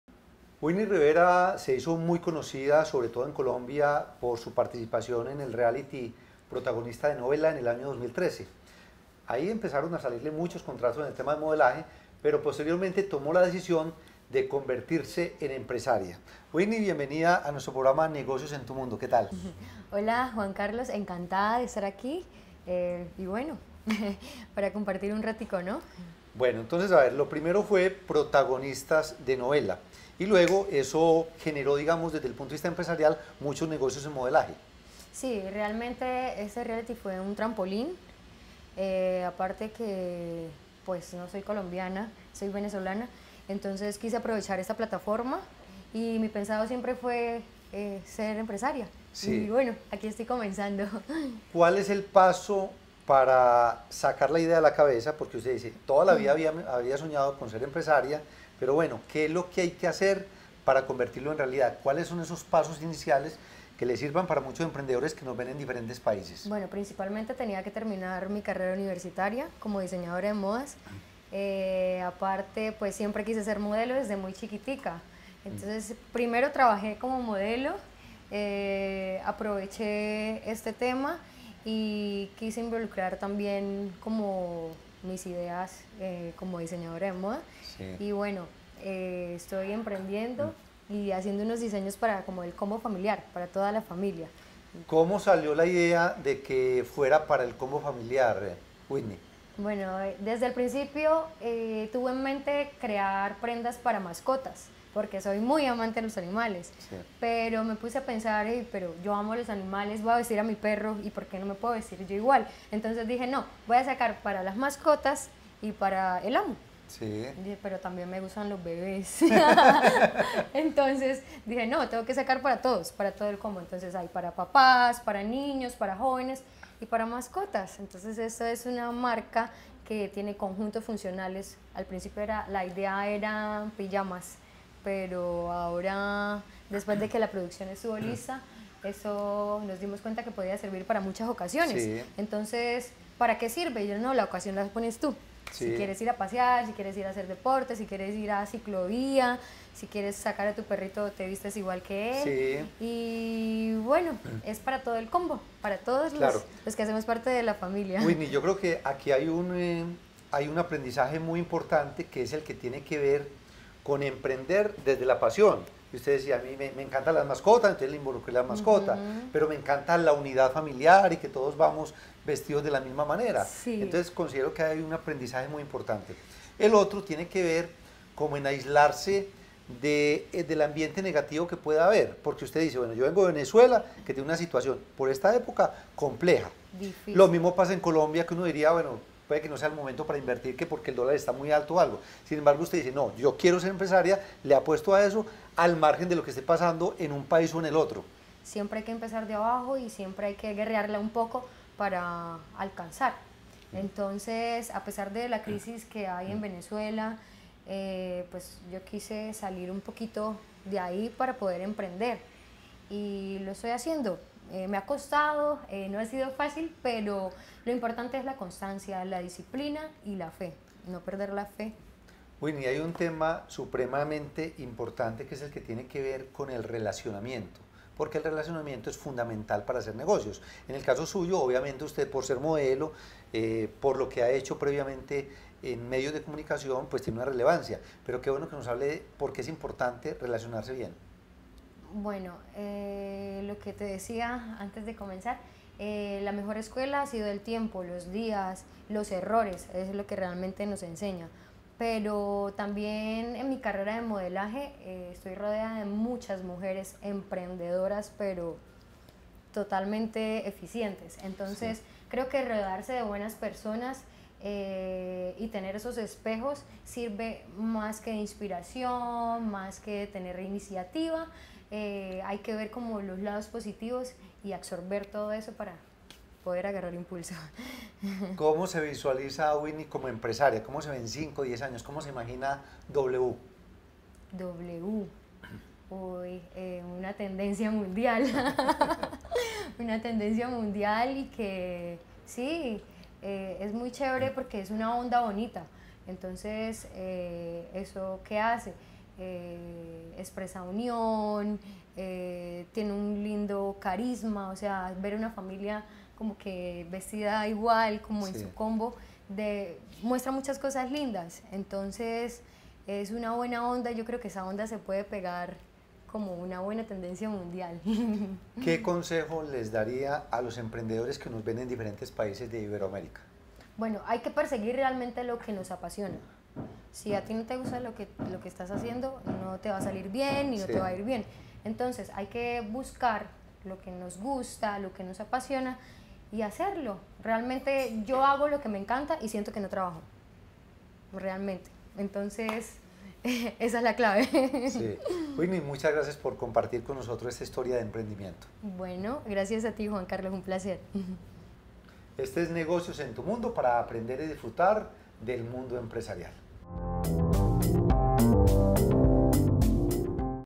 "Emprender desde la pasión", la historia de emprendimiento de Whitney Rivera.
Nuestro coach y empresario Juan Carlos Yepes (@Juancarlosy) conversa con  Whitney Rivera, quién es ex participante del reconocido reality show Colombiano Protagonistas de Novela, y en esta ocasión nos habla acerca de su faceta como empresaria con su marca “W”.

Whitney Rivera participó en el Reality Show Protagonistas de Novela en la emisión del año  2013, la cual resultó un trampolín para su carrera de modelaje y para lo que hoy en día es su faceta como empresaria con su marca de ropa para toda la familia.

La idea de esta marca de ropa fue originalmente pensada para mascotas, puesto que la diseñadora es fiel amante de los animales y pensó en la posibilidad de crear un atuendo que combinara con lo que el dueño lleve puesto.  Partiendo de estas ideas nace su emprendimiento que comenzó siendo para las mascotas y terminó siendo para toda la familia.